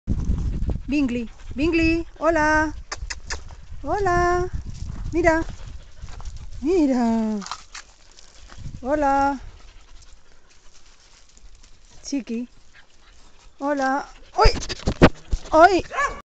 Bingli,Chiqui (vermittelt)
Diese Hunde warten in Almeria auf ein Zuhause.